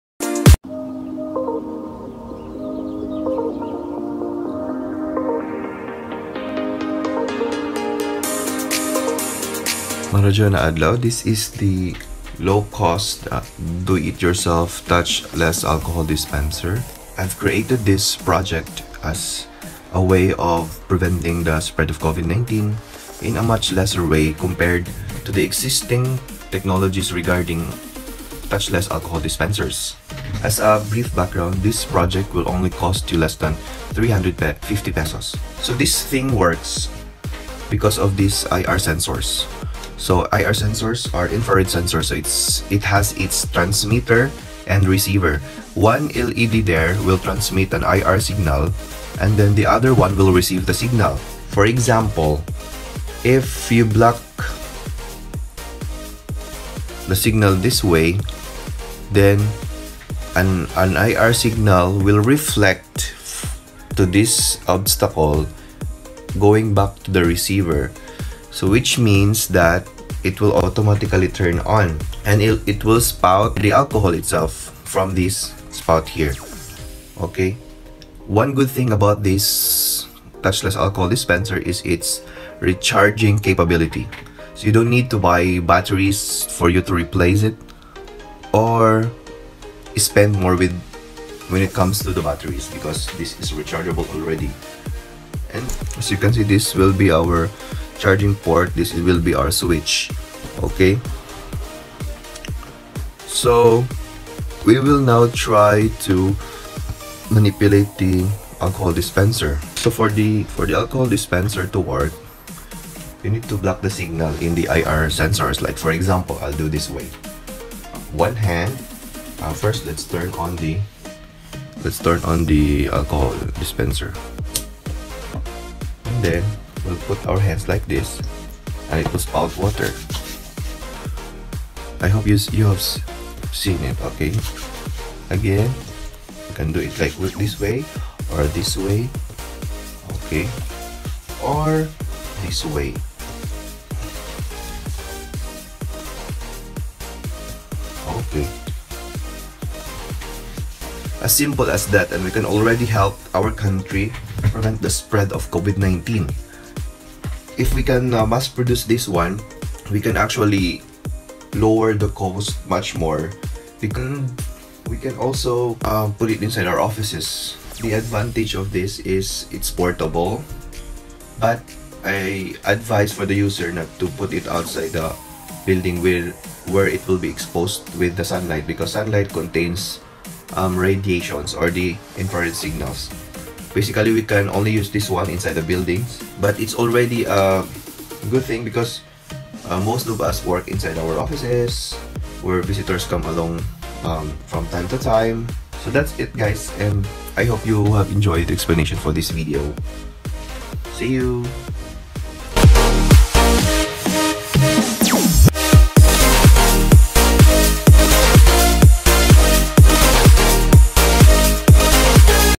Touchless Alcohol Dispenser | No Arduino Needed
This is a project by Engr. Gideon G. Buniel also known as "G" that participated in the recent Regional-wide Online Makers Challenge of Department of Science and Technology (DOST) Caraga under the non-student category.  The said competition is for these inventors and innovators to bring interventions or to outsmart covid-19. This project does not need Arduino microcontrollers because of the TIP42C transistor which serves as the switch for this project that also controls the Infrared (IR) proximity sensor. If it detects an obstacle within 5cm distance, the water pump turns-on which spout alcohol.

This is TAD: A Low-cost Do-It-Yourself Touchless Alcohol Dispenser.

You can message me if you want to know more about this product or if you want to learn something about this.